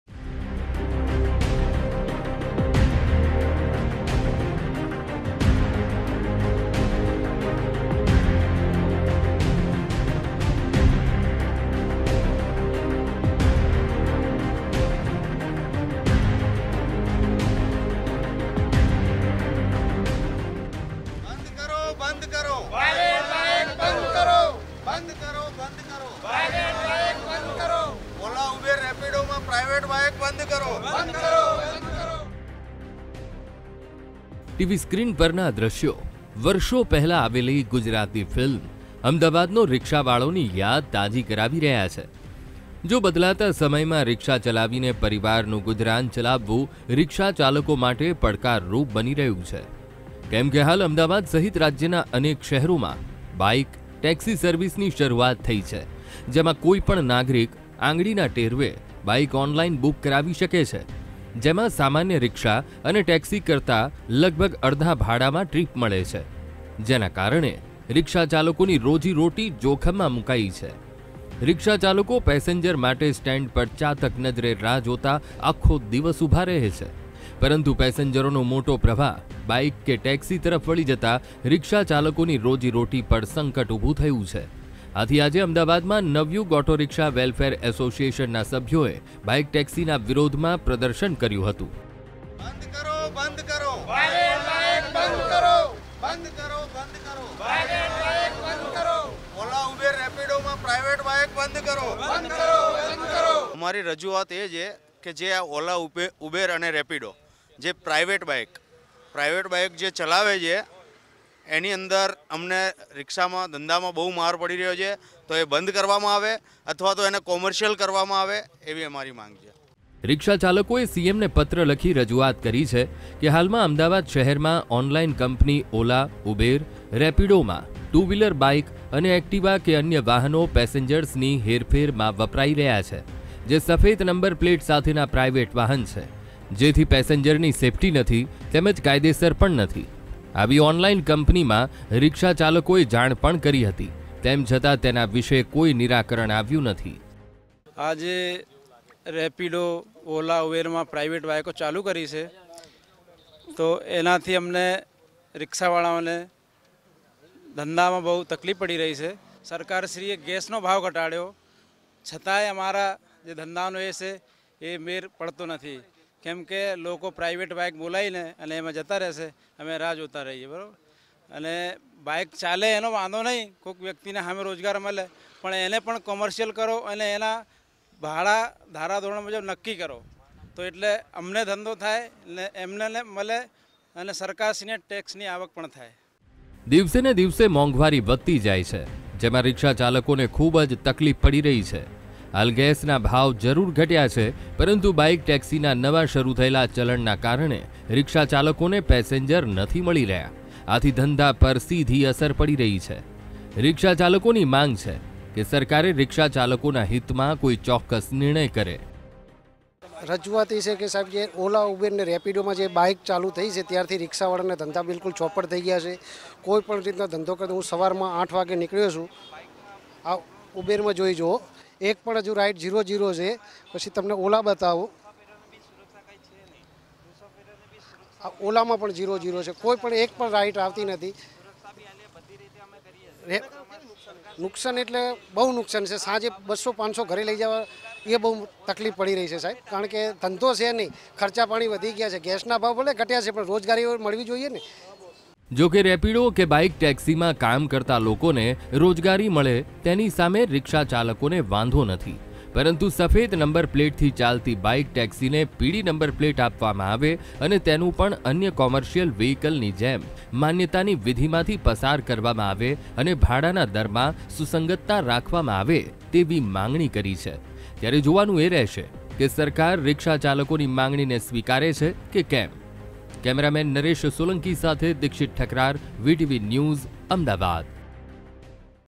ઓનલાઇન બુકિંગમાં રિક્ષા અને ટેક્સીકાર કરતા લગભગ અડધા ભાડામાં મળે છે, રિક્ષાચાલકોની રોજીરોટી પર સંકટ
હાલ અમદાવાદ સહીત રાજ્યના અનેક શહેરોમાં બાઈક ટેક્સી સર્વિસની શરૂઆત થઇ છે. જેમાં કોઈપણ નાગરિક આંગળીના ટેરવે બાઈક ઓનલાઇન બુક કરાવી શકે છે.. જેમાં સામાન્ય રિક્ષા અને ટેક્સીકાર કરતા લગભગ અડધા ભાડામાં ટ્રીપ મળે છે, પરંતુ તેના કારણે રિક્ષાચાલકોની રોજીરોટી પર સંકટ ઊભું થયું છે...શું કહેવું છે રિક્ષાચાલકોનું જોઈએ